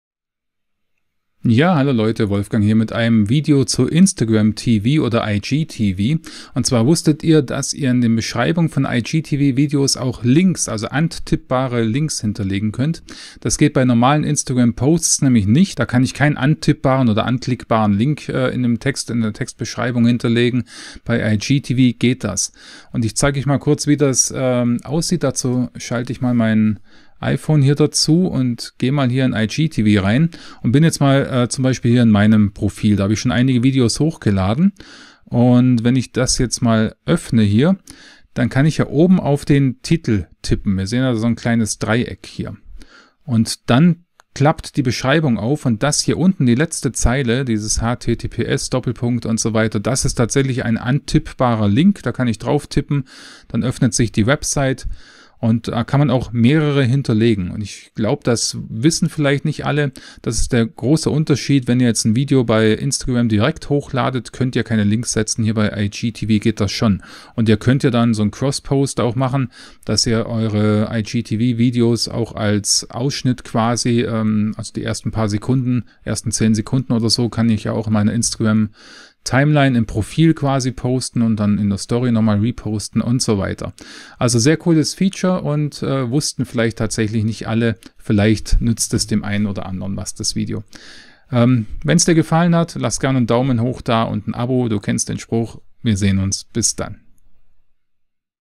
Anklickbare Links in der Beschreibung von IGTV Videos unterbringen
Im Beschreibungstext normaler Instagram-Posts kann man keine anklickbaren Links hinterlegen. In der Beschreibung von IGTV Videos geht das aber schon. Wie das dann aussieht, zeige ich in diesem Video.

Du möchtest persönlichen und direkten Support bei Fragen rund um TYPO3 oder die Videokurse? Dann schau dir mein Angebot an
JETZT ERHÄLTLiCH: Das TYPO3 9 LTS Videotraining